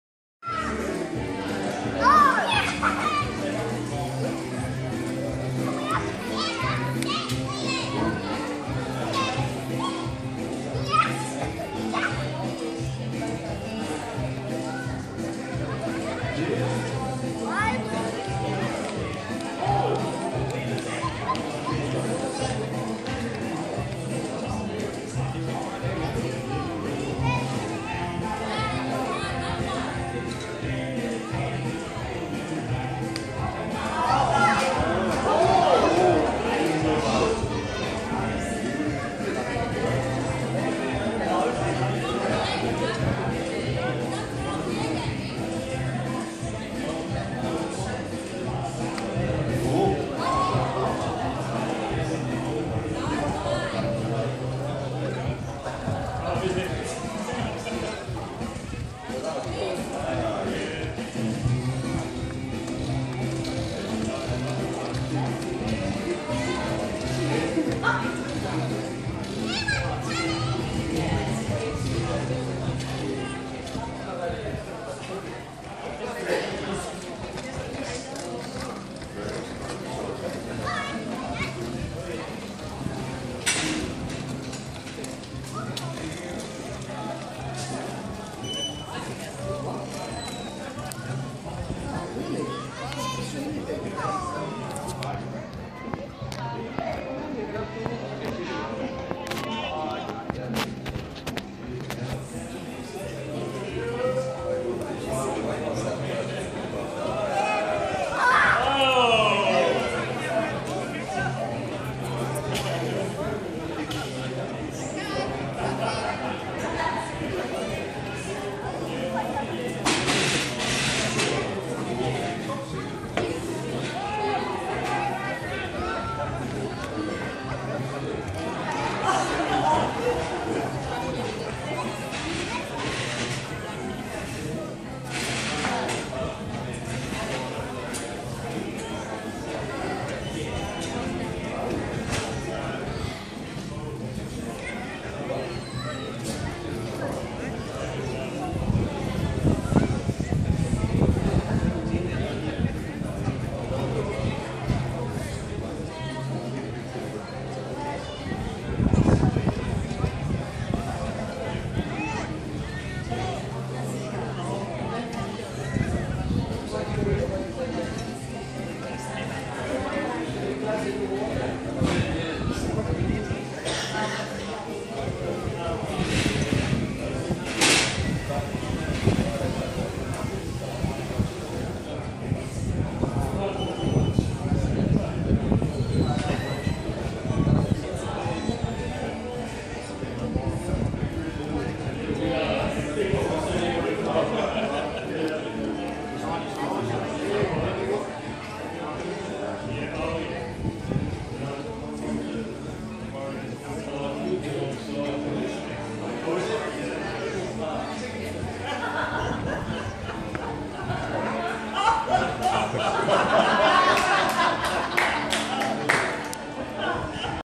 Australia Day at the RMYC
Backyard cricket takes a different twist when the back yard is all water